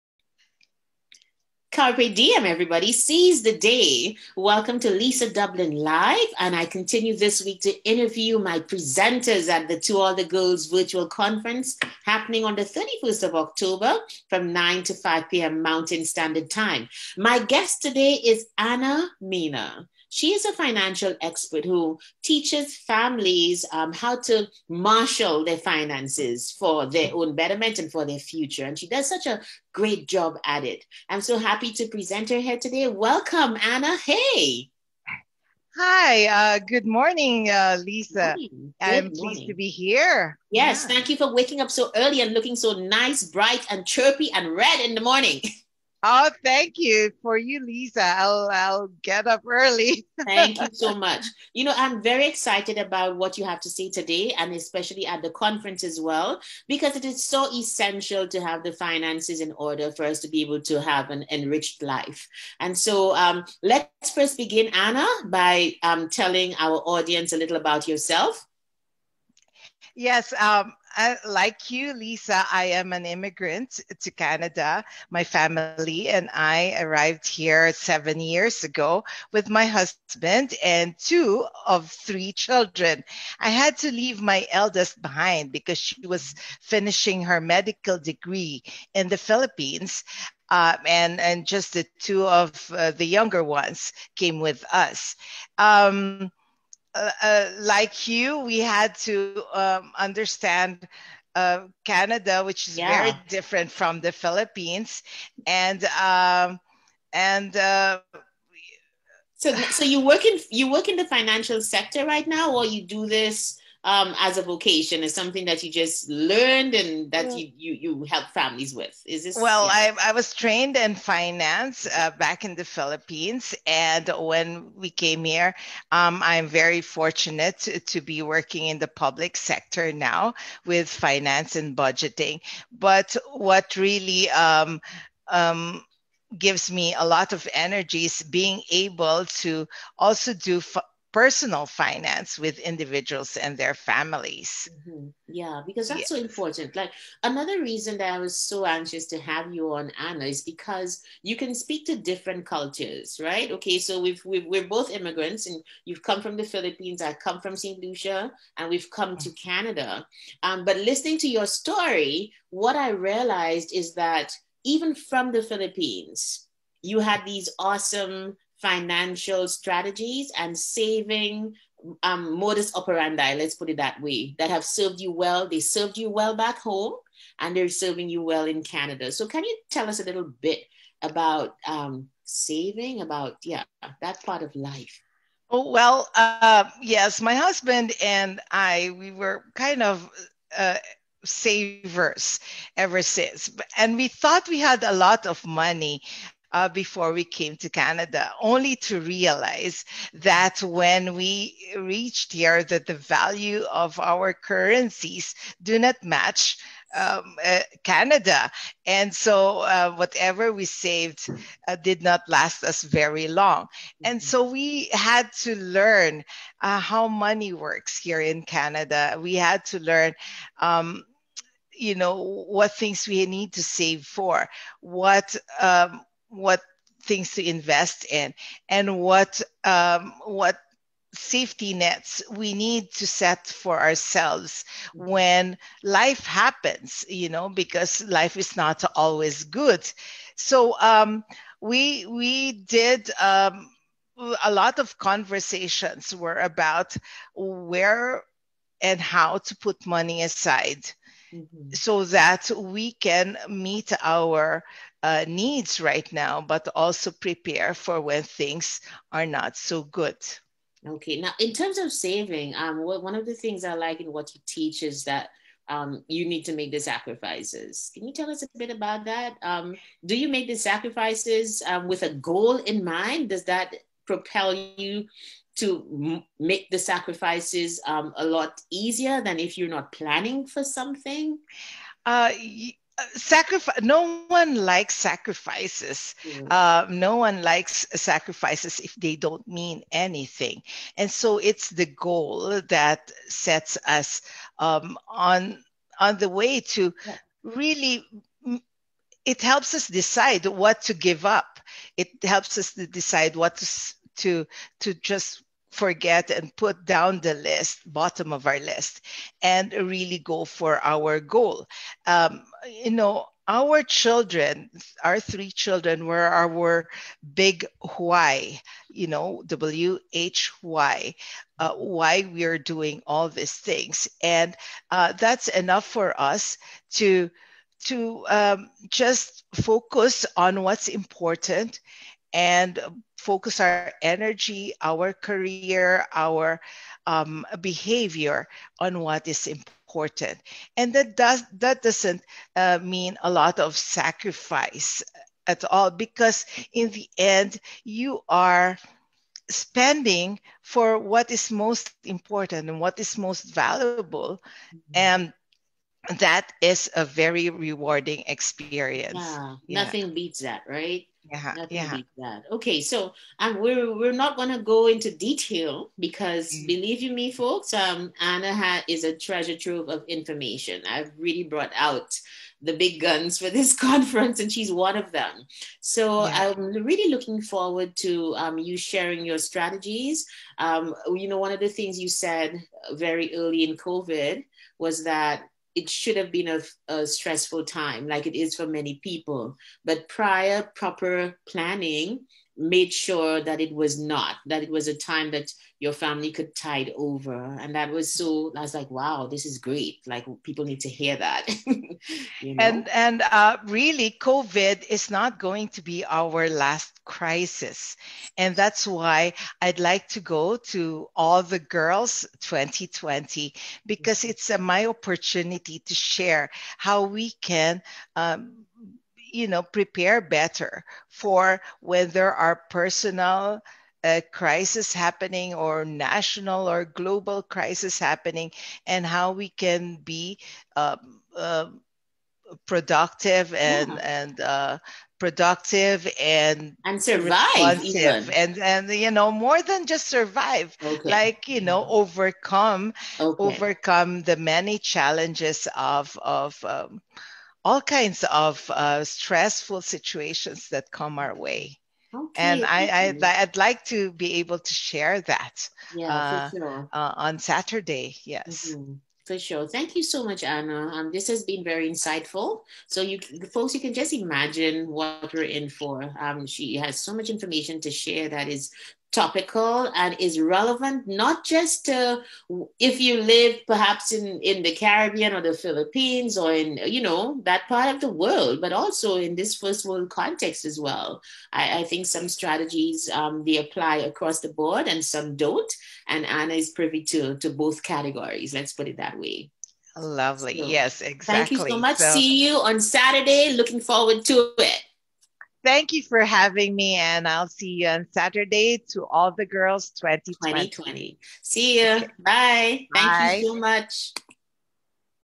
Lisa Dublin, founder of TATG, interviews family finance expert Anna Mina
Anna Mina is a Filipino-Canadian whose specialty is financial literacy for families. She will be presenting at TATG20. Anna not only helps families, she lives what she teaches.